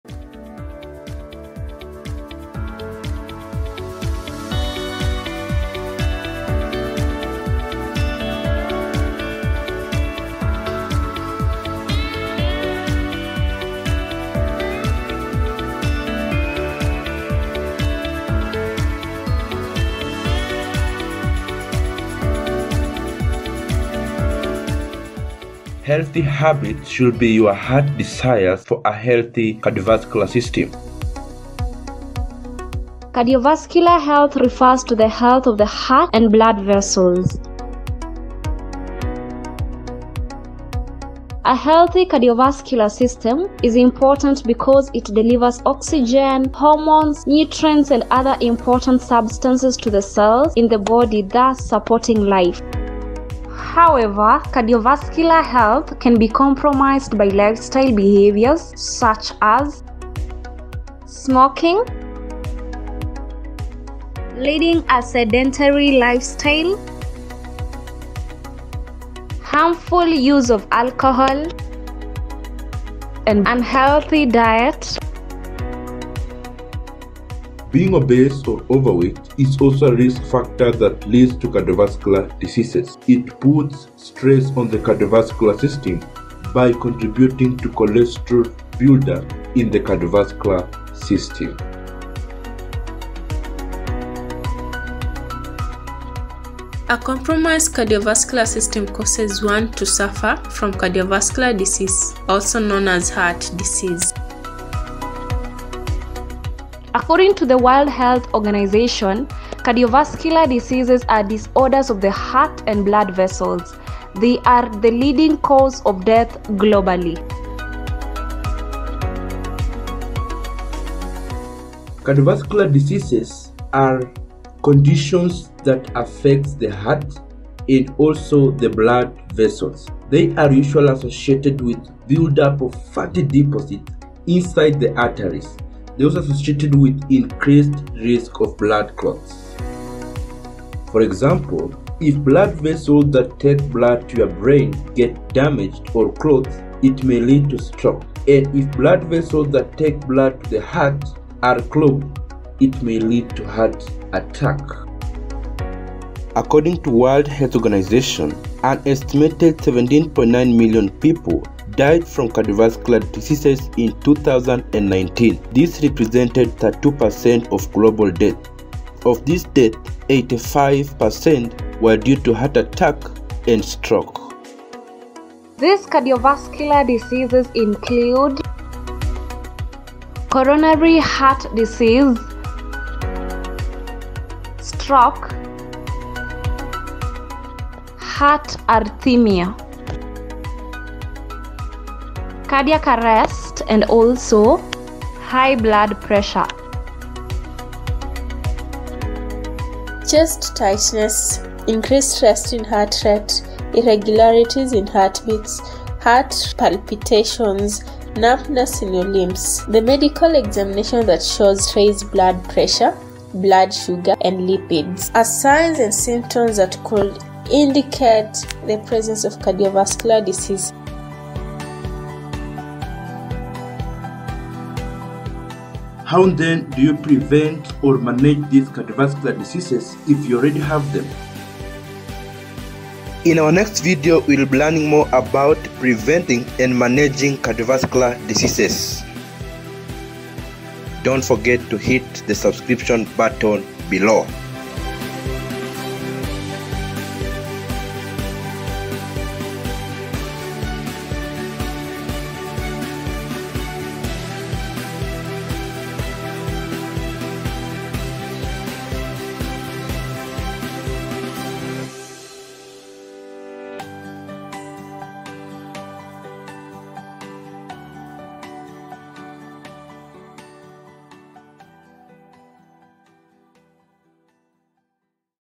How to Prevent or Manage Cardiovascular Diseases Part 1 (What are cardiovascular diseases)
This video (part one of How to prevent or manage cardiovascular diseases) has explained; the definition of the cardiovascular system and cardiovascular diseases, the risk factors, and examples of cardiovascular diseases. How cardiovascular diseases are caused and the sign and symptoms that could indicate the presence of cardiovascular diseases.

Don"t forget to watch parts 2 and 3. They will give us the best means towards preventing and managing cardiovascular diseases.